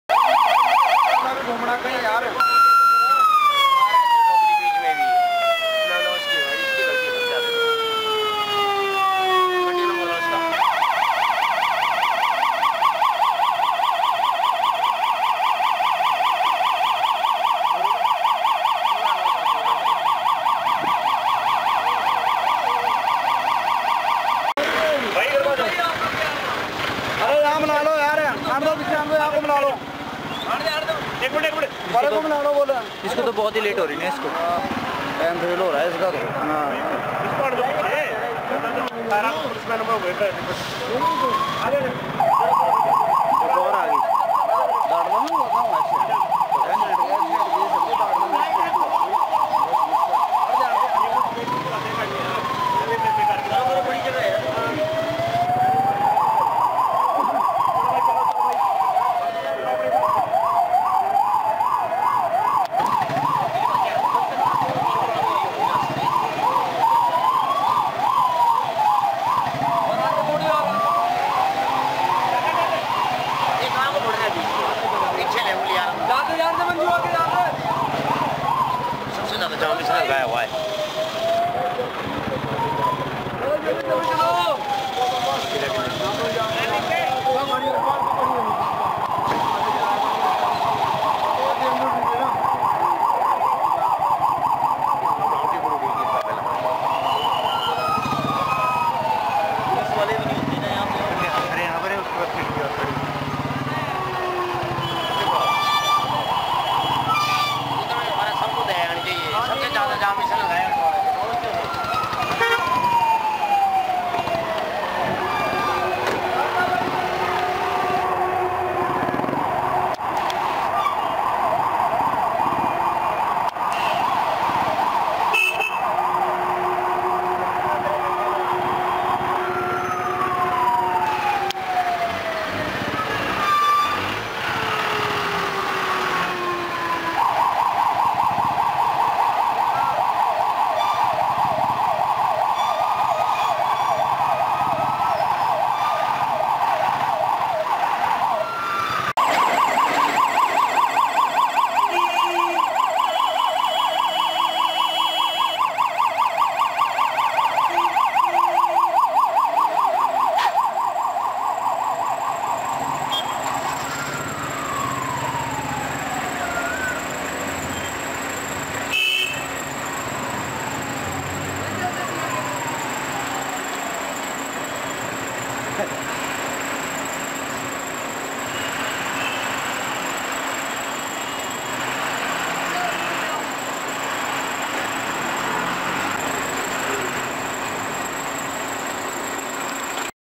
Ambulance struck in Traffic Jam at Boileauganj Chowk for over 10-15 minutes
Due to the insensitivity of Shimla's drives and negligence of Shimla traffic police, the 108 ambulances carrying seriously injured victims of the truck accident near Darlaghat, remained stuck in traffic for about 10-15 minutes.